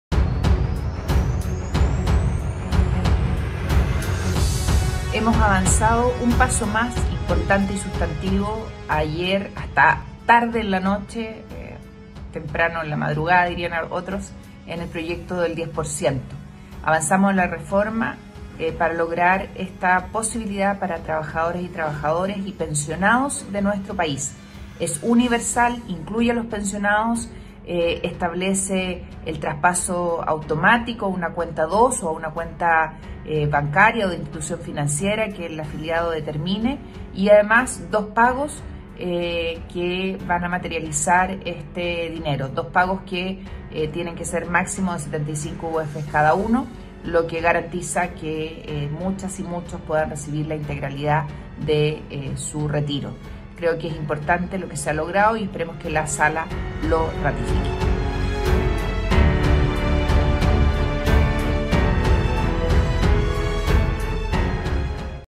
Senadora Rincón: “Dimos un paso importante y sustantivo para retiro del 10 por ciento”
Parlamentaria por el Maule valoró aprobación en general del proyecto en la comisión de Constitución, Legislación y Justicia, donde también se votaron indicaciones para mejorar la iniciativa. “Se garantiza universalidad, se aclara que pago se hará en dos cuotas con tope máximo de 75 UF cada una a la cuenta dos o a la de institución financiera que el afiliado determine”, explicó
Como un paso importante y sustantivo para hacer realidad el retiro del diez por ciento, calificó la Senadora por el Maule, Ximena Rincón, la aprobación en general y de indicaciones a esta iniciativa en la comisión de Constitución, Legislación y Justicia, tras lo cual el proyecto quedó en condiciones de ser votado en sala este miércoles 22 de julio.